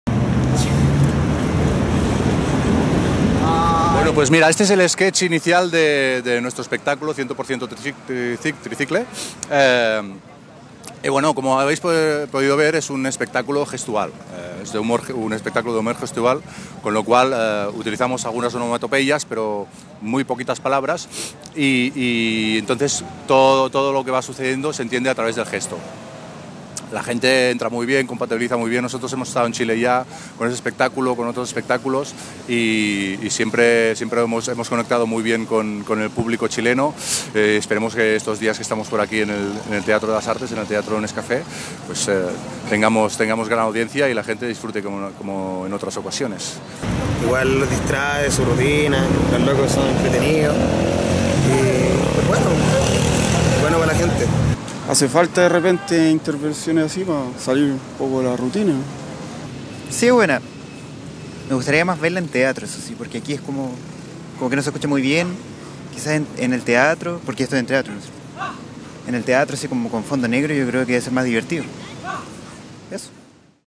Humoristas españoles hicieron intervención en Providencia
Afuera del metro Salvador la compañía catalana de humor clown "100% Tricicle" simuló un partido de tenis, uno de sus mejores shows.